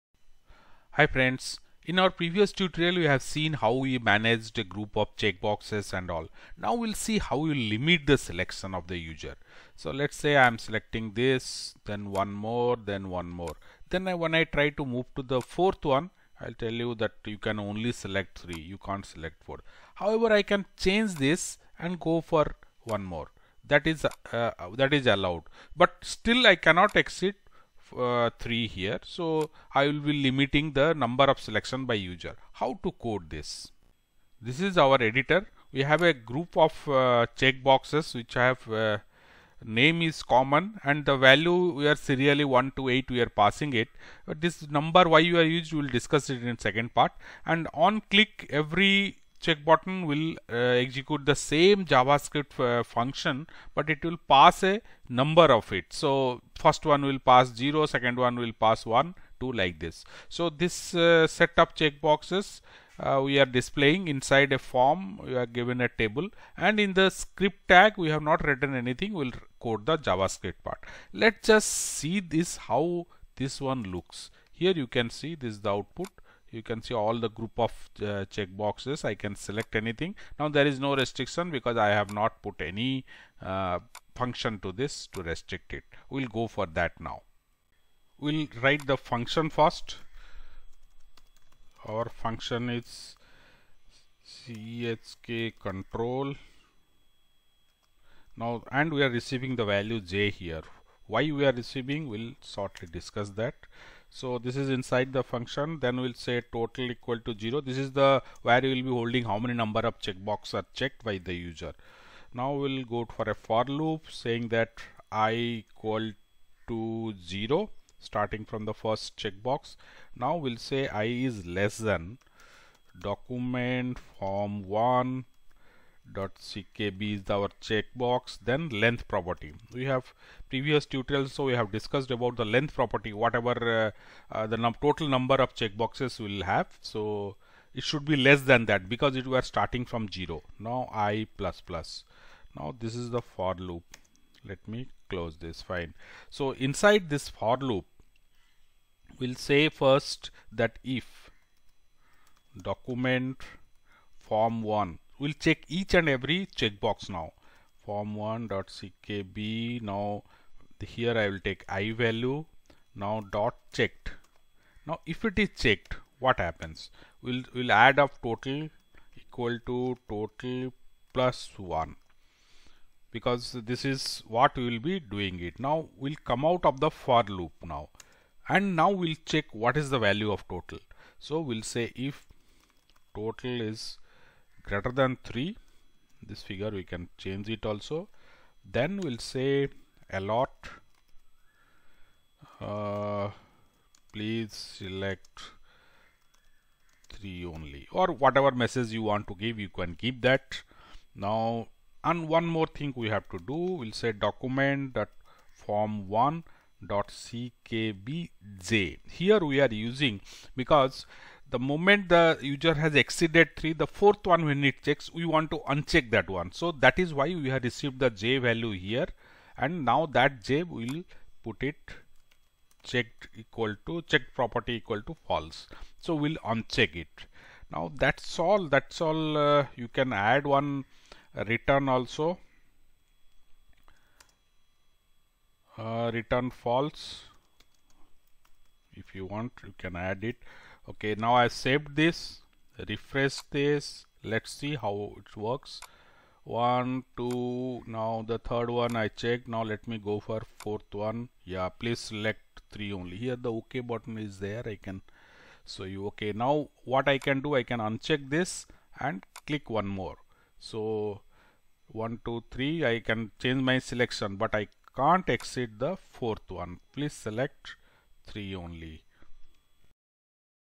Limiting number of selection user can check from a group of checkboxes by using JavaScript
We can restrict or fix one upper limit for the user to select checkboxes from a set of checkboxes. After reaching upper limit, user can unselect any selection and go for selecting others but the upper limit will be maintained.
All checkbox will have common name and onClick event of the checkbox will trigger one JavaScript function chkcontrol(). This function will also receive the last selected number of the checkbox.
Inside this function we will first read the status ( selected or not ) from all the checkboxes and increase the value by 1 for each selected checkbox. By doing this we will come to know about the total number of checkboxes selected by the user.
if(document.form1.ckb[i].checked){
total =total +1;
After knowing the total number of selections we will use one if condition to check the value and if it is more than our set limit then we will display one alert box asking the user to restrict the number of selection to our set limit.
Now we have to uncheck the last selection by user by using the parameter we received ( j ) as input to the function. This is required as we are not in a position to exceed our limit. We will use checked property of the checkbox to unselect it.